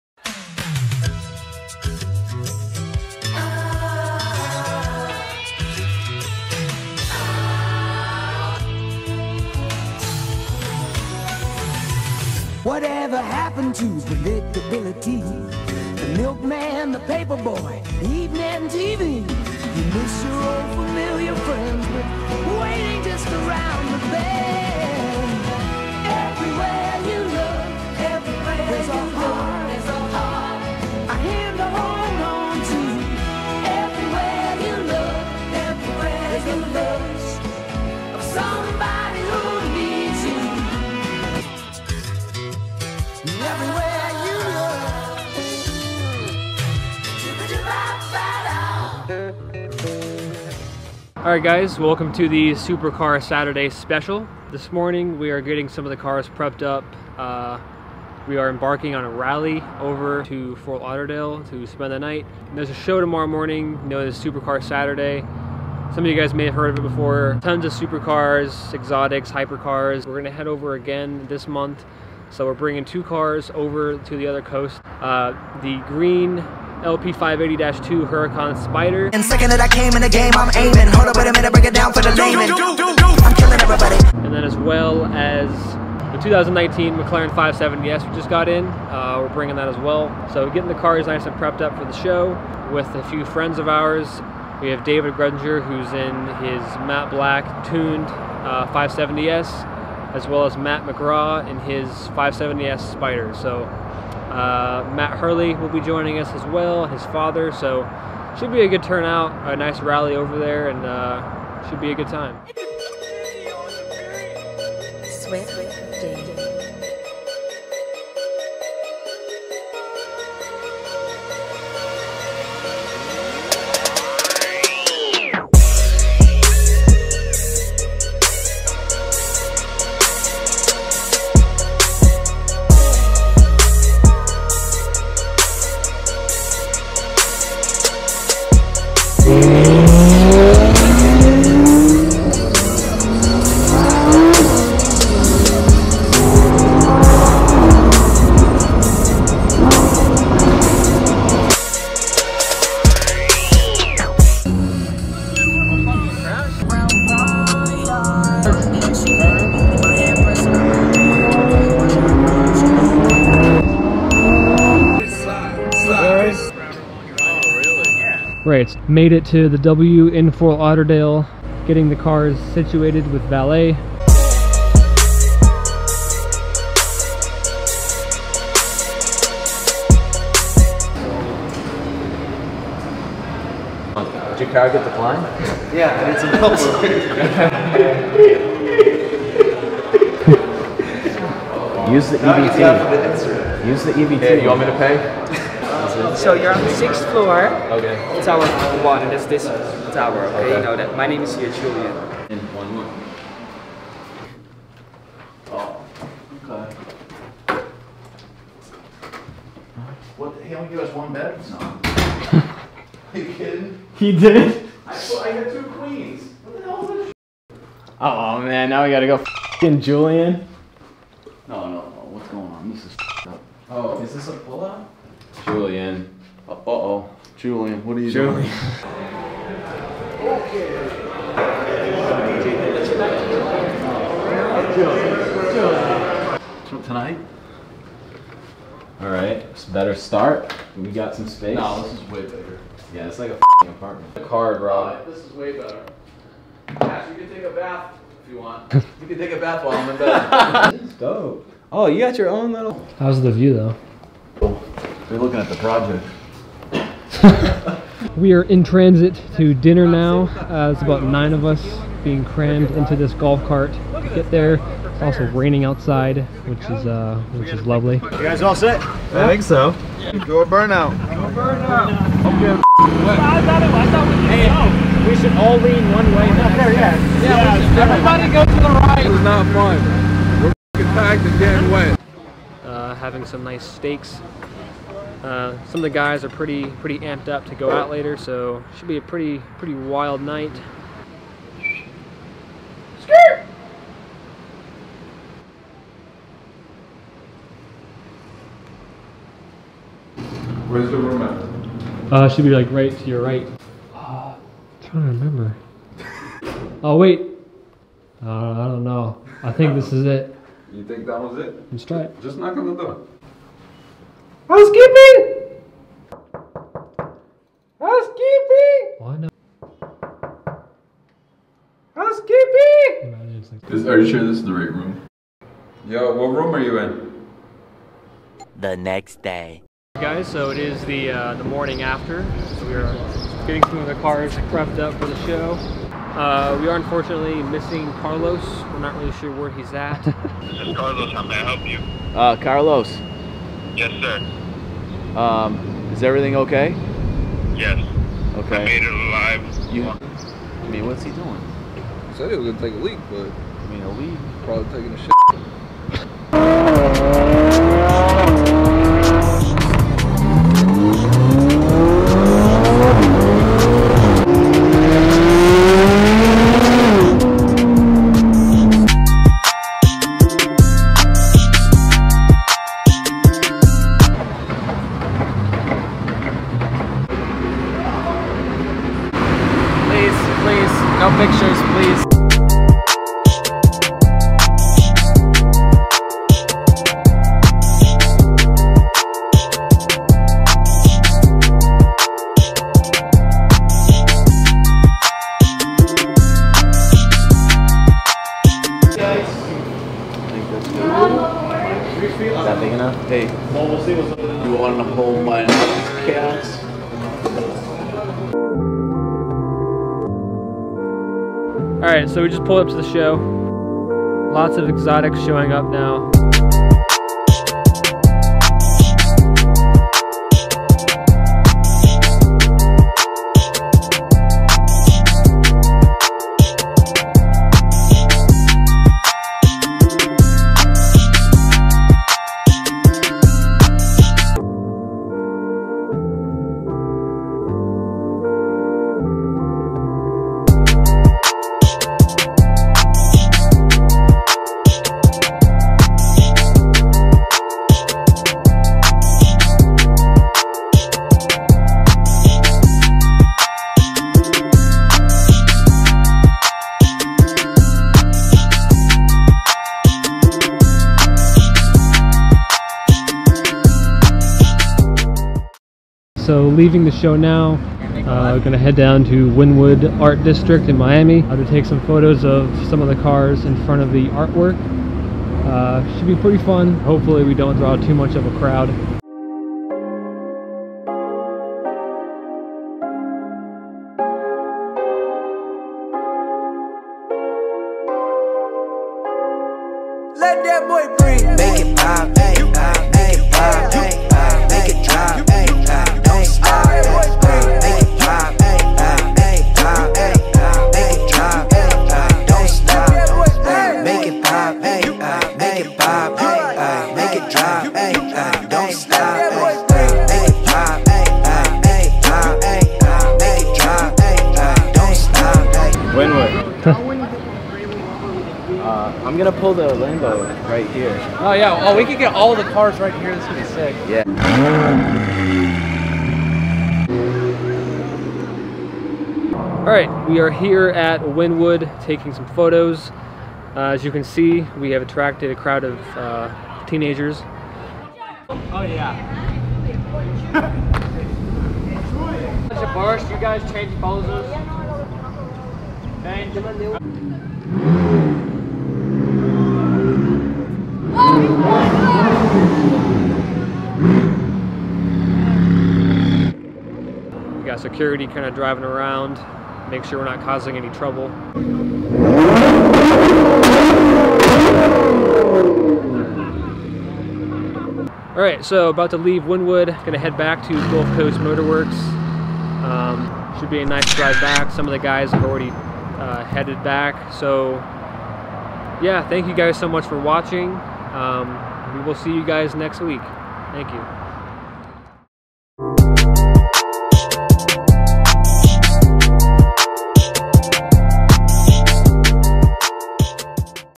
Season 2 Episode 3 | Supercar Saturday Special
NEW VLOG!
The Supercar Saturday Special! In this episode, the team rallies over to Fort Lauderdale for the monthly Supercar Saturdays Florida car show, as well as to Wynwood Art District in Miami for a photoshoot.